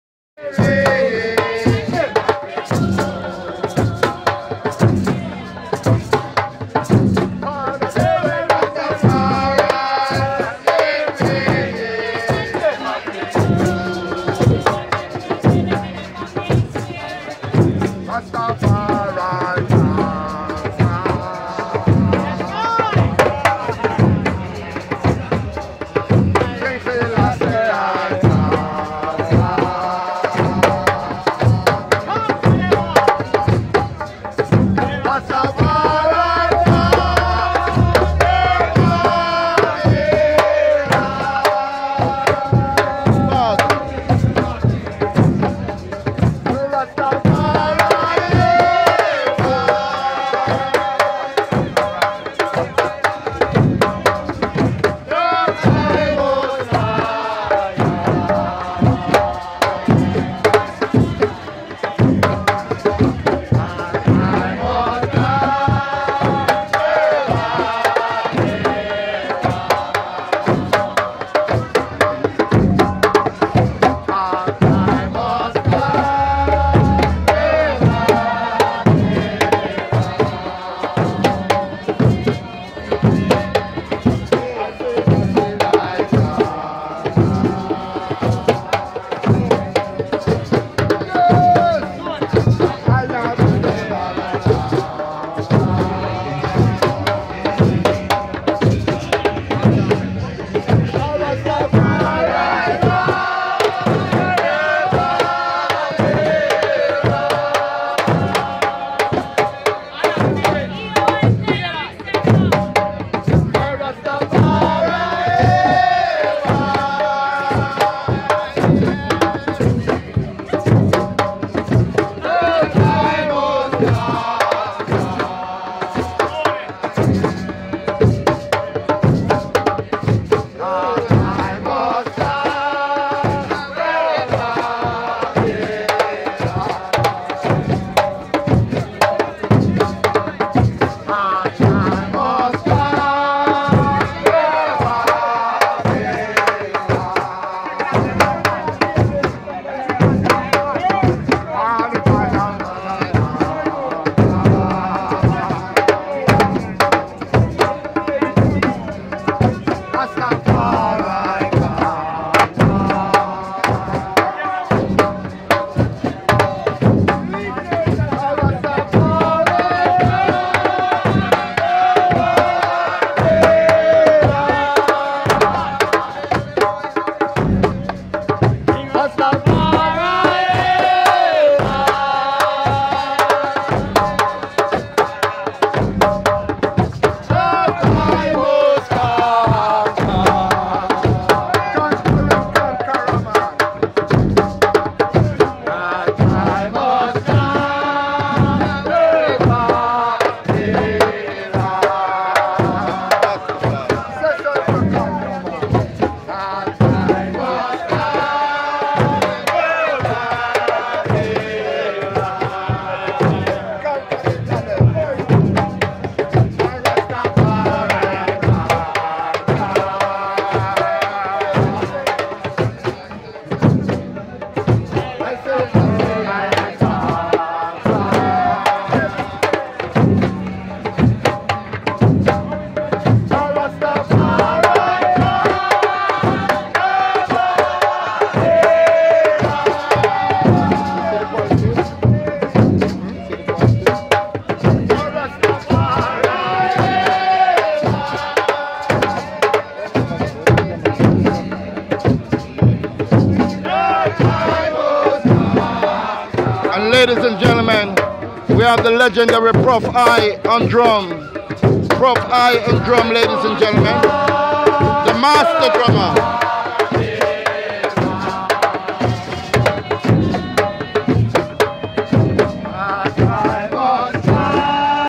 VETERAN DRUMMER PROF I WAS AWESOME IRIE FM FAKE BUST RALLY
Veteran drummer Prof I was awesome playing the drums at Papine Square to remove the fake bust from the UWI